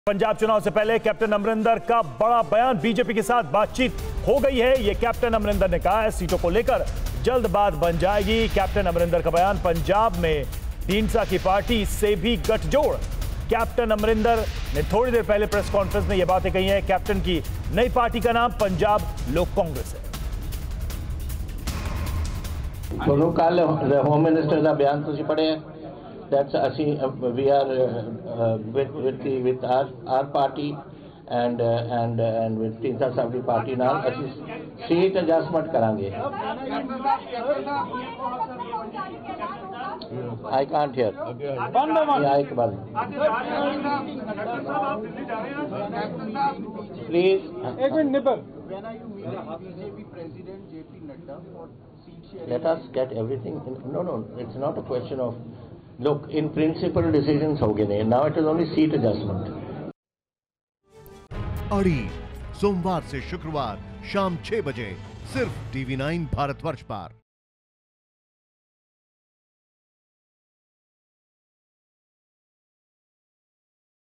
Captain Amarinder Singh का ऐलान- BJP और Shiromani Akali Dal के साथ लड़ेंगे चुनाव
पंजाब लोक कांग्रेस (Punjab Lok Congress) प्रमुख कैप्टन अमरिंदर सिंह ने अपने नए पार्टी दफ्तर से बड़ा ऐलान किया है. उन्होंने कहा कि बीजेपी से उनकी बातचीत हो गई है और जल्द ही सीट शेयरिंग फॉर्मूले पर पिक्चर क्लियर होगी. साथ ही उन्होंने कहा, दो पार्टियों के साथ सीट अरेंजमेंट पर बात हो रही है.